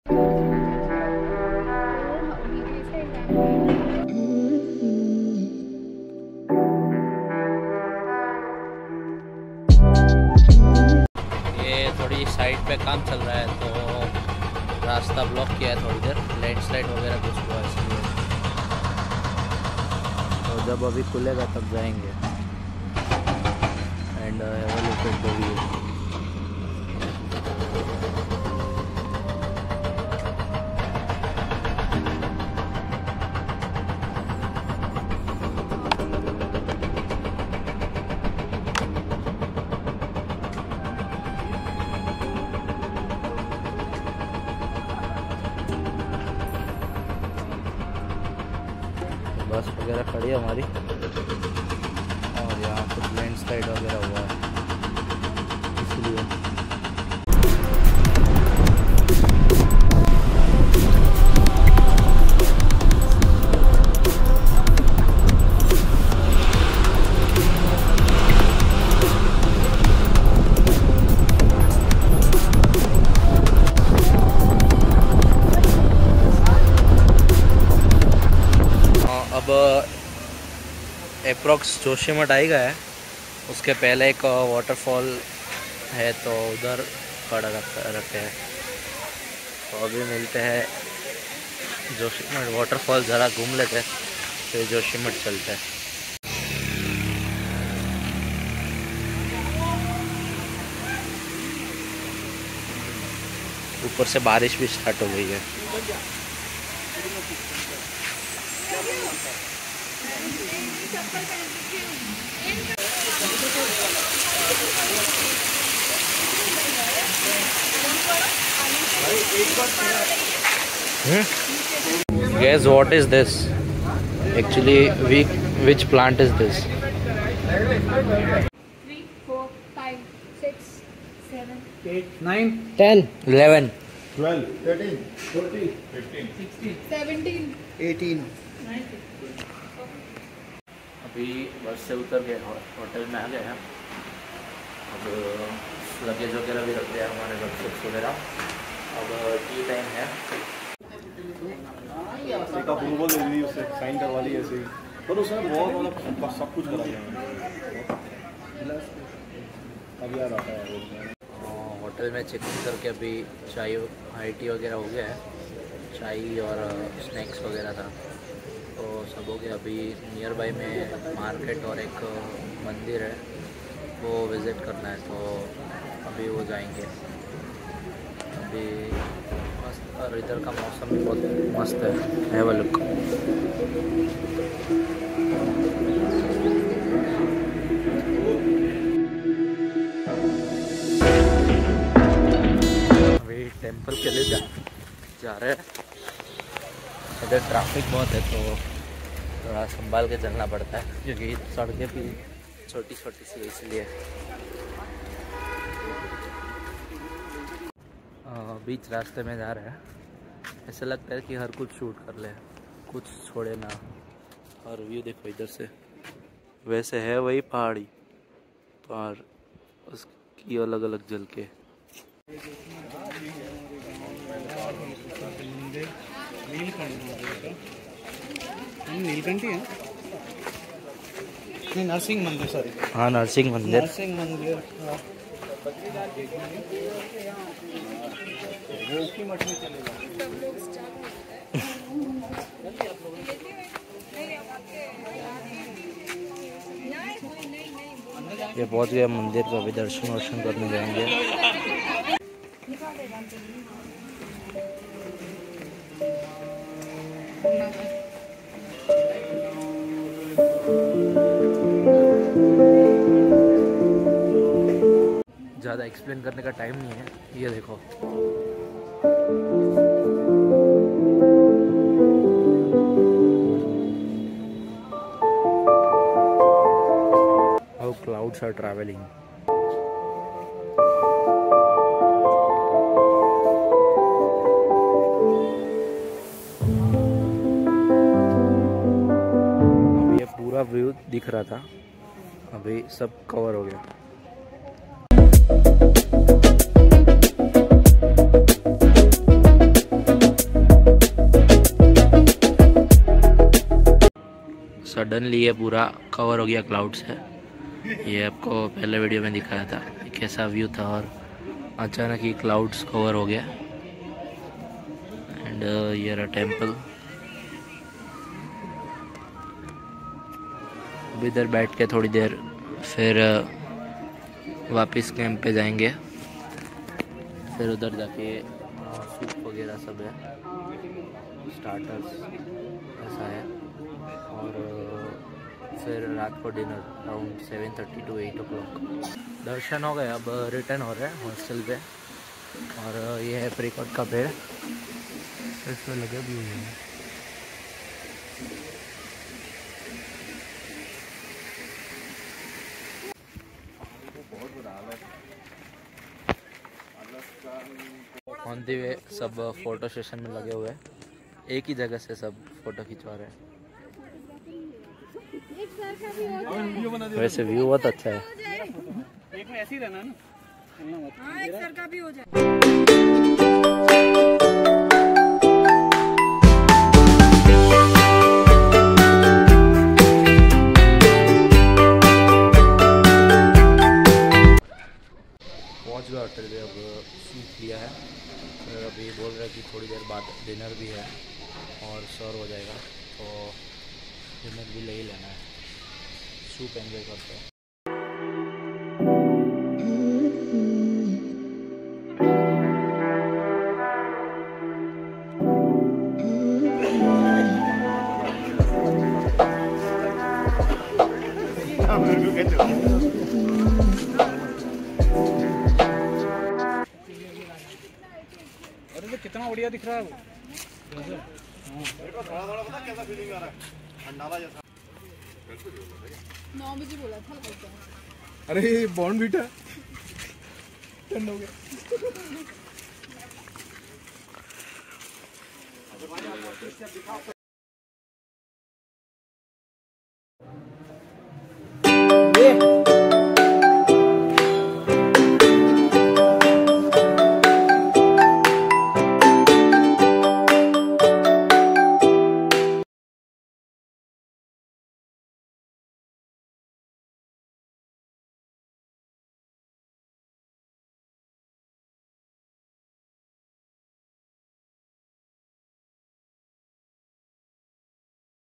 Places to visit in Joshimath | Narshing temple | Joshimath market | Vlog 2 | Part 2
This is a video on series valley of flowers and our stay at Joshimath. and roaming around in Joshimath, Joshimath is a nice small town which has a beautiful sceneries with coming and peaceful and hotels

Narshing temple Joshimath
joshimath temple
joshimath market
places to visit in Joshimath
hotels in Joshimath
exploring joshimath